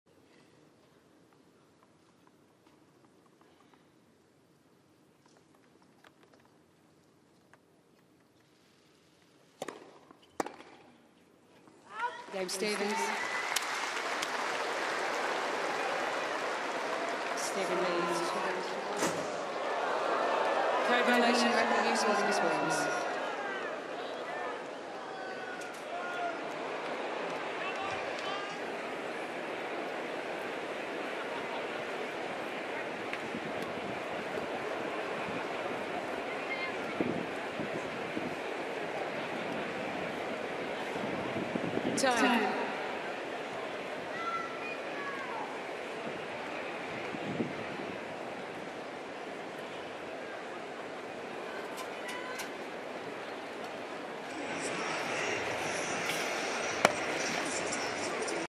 Serena Williams Destroys Racquet | Australian Open 2013
Locked in a tight battle with Sloane Stephens, Serena Williams takes out her frustration on her racquet.

Australian Open 2016 from 19 January - 1 February 2016.